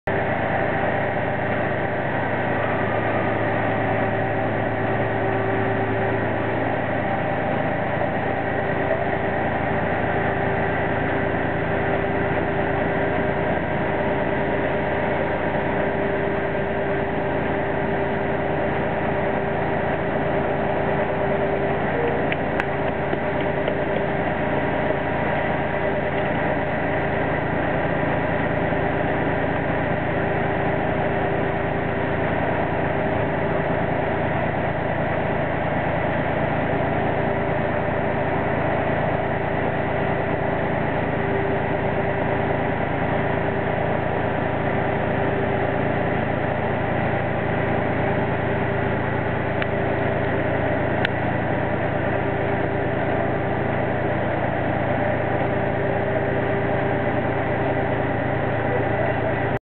Salford Clermont Square - Manchester Spinningfields footbridge
Sunday 29 January 2012 the new footbridge lifted in position